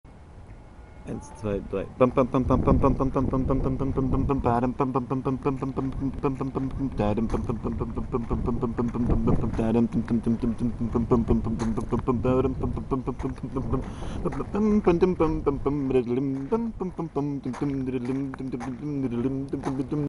the swiss hard box idances in the pantheon in athens
a sirtaki in athens by the beautiful swiss hard box dedicated to its apple iphone.